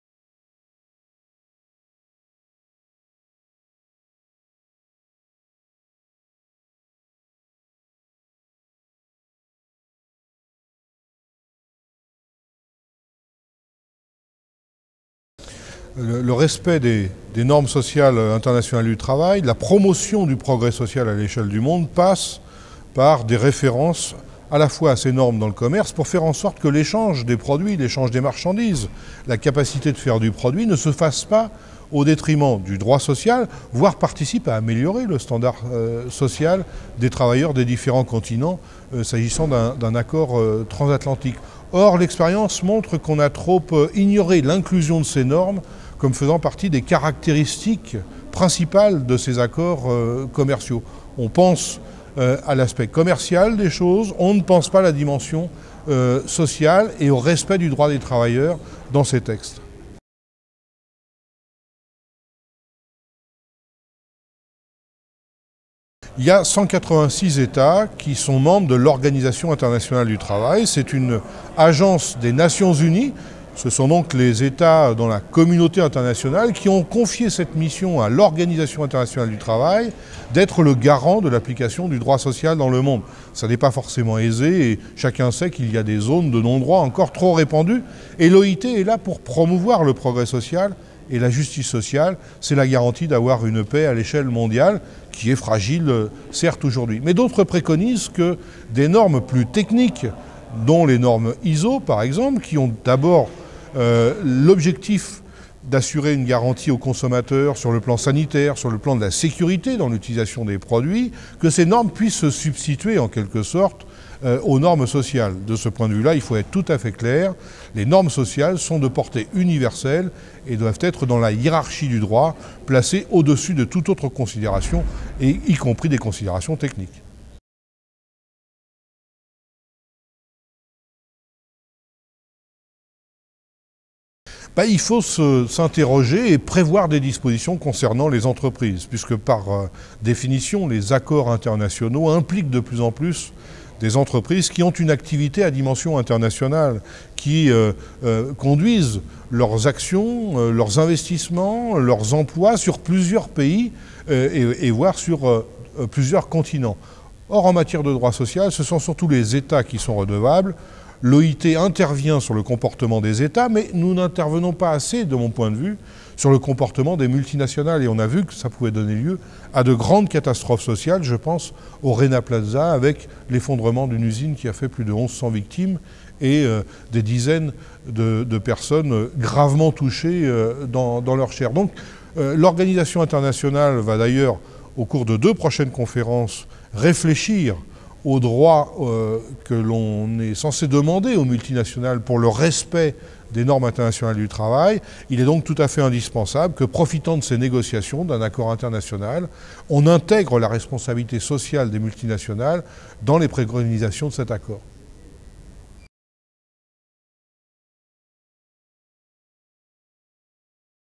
Interview de Bernard THIBAULT, Organisation Internationale du Travail - cese
Interview de M. Bernard THIBAULT, membre du Conseil d’Administration de l’Organisation Internationale du Travail (OIT), entendu par la Section des affaires européennes et internationales du CESE dans le cadre de la saisine gouvernementale : "Les enjeux de la négociation du projet de partenariat transatlantique pour le commerce et l’investissement (PTCI)".